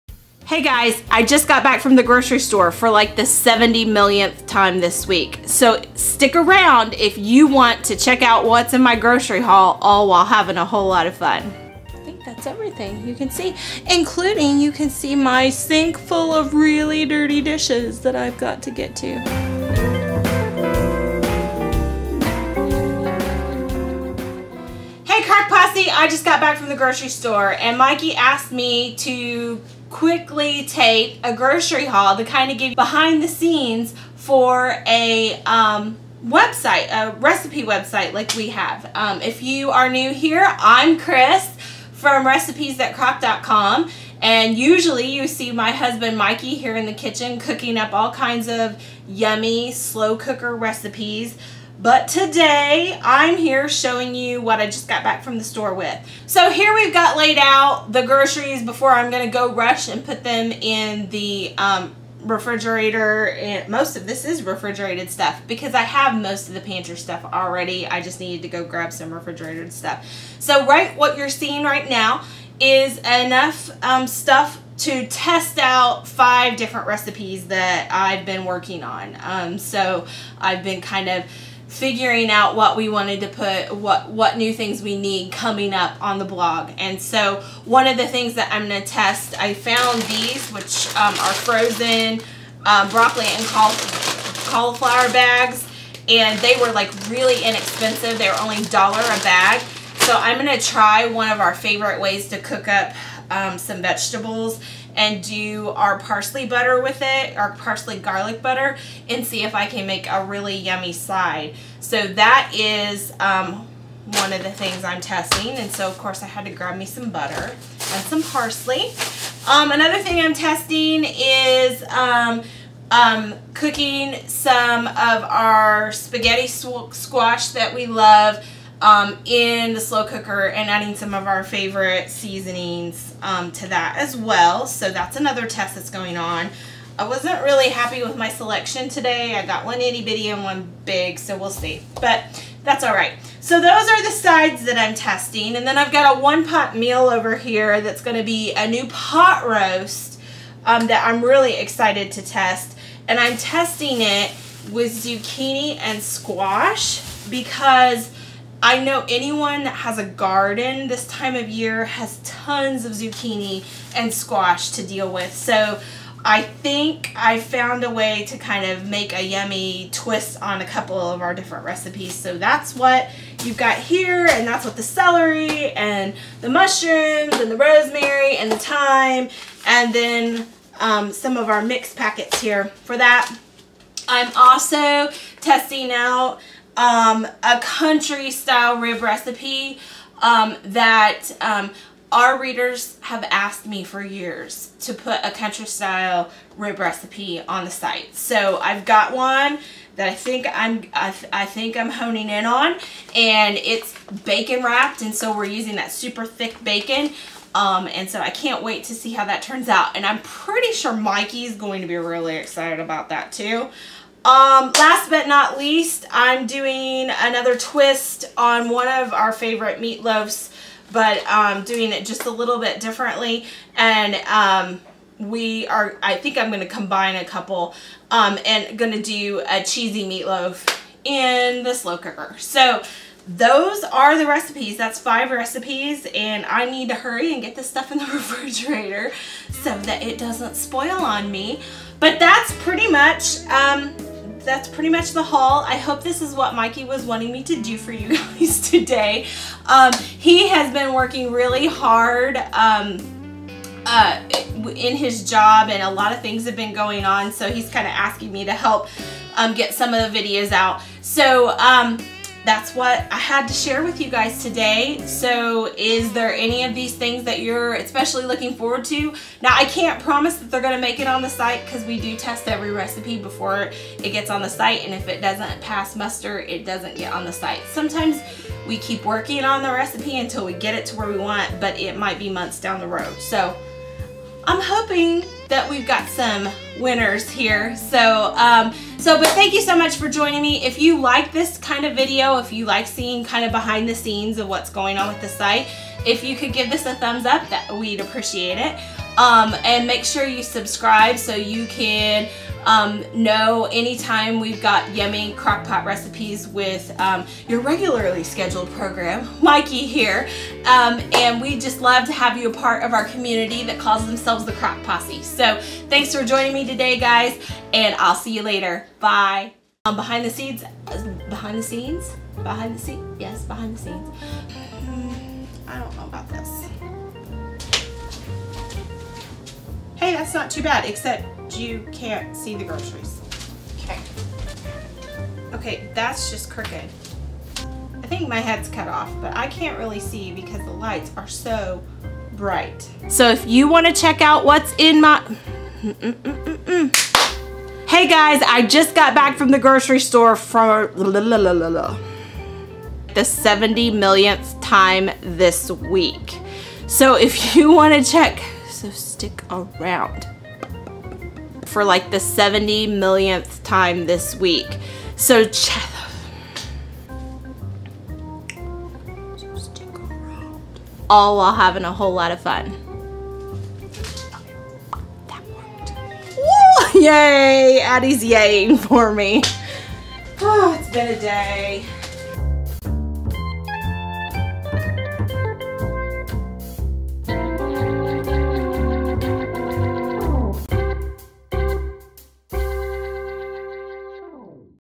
Grocery Haul: Crock Pot Recipe Testing
Cris is sharing a grocery haul for some of the crock pot recipes she is testing for Recipes That Crock. Check out this video if you want a sneak peek behind the scenes!